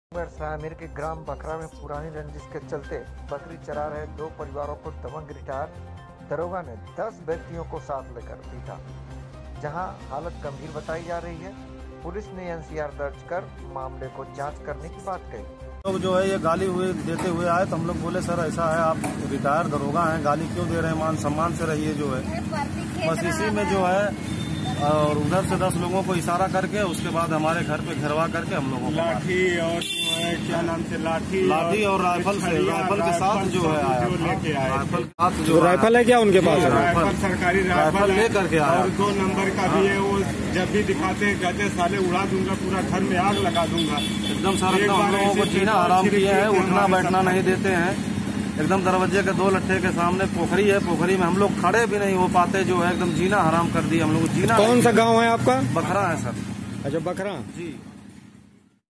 दो परिवारों को दबंग रिटायर दरोगा ने 10 व्यक्तियों को साथ लेकर पीटा हालत गंभीर
Aajkichaallivenews